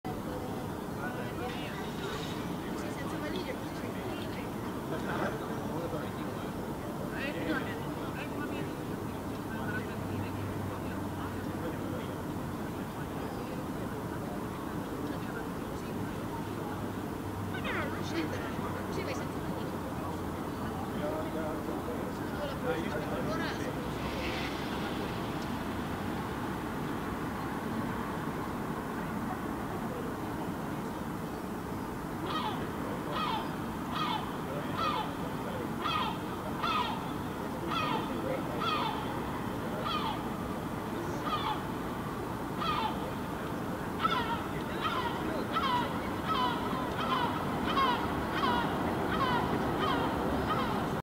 Herring Gull keeping an eye on its fledgling, vocalizing
Herring Gull (Larus argentatus) parent standing on the rooftop, keeping an eye on its fledgling; vocalizing, taking off.

Harmaalokkiemo (Larus argentatus) seisoo talon katolla, pitää silmällä maastopoikastaan; ääntelee, lähtee lentoon.